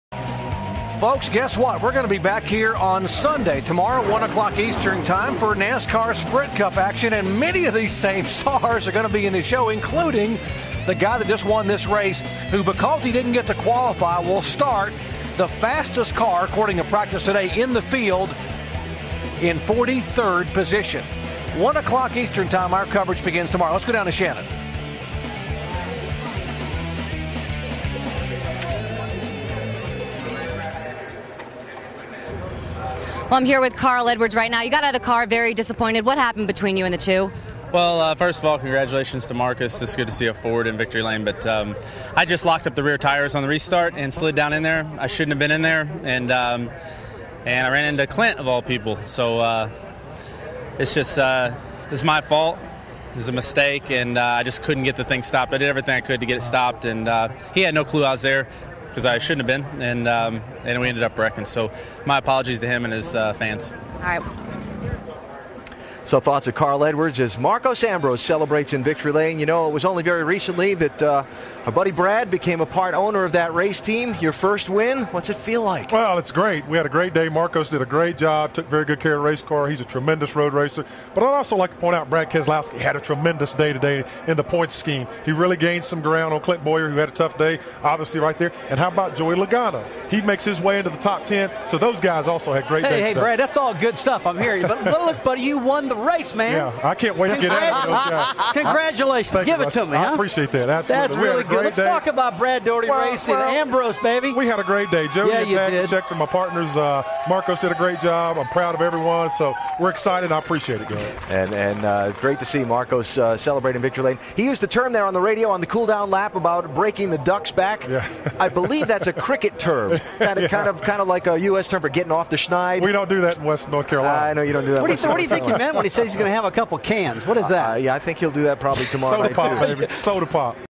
NNS Zippo 200 Watkins Glen - Victory Lane - Carl Edwards
Marcos Ambrose Victory Lane wrap up and Shannon Spake interviews Carl Edwards.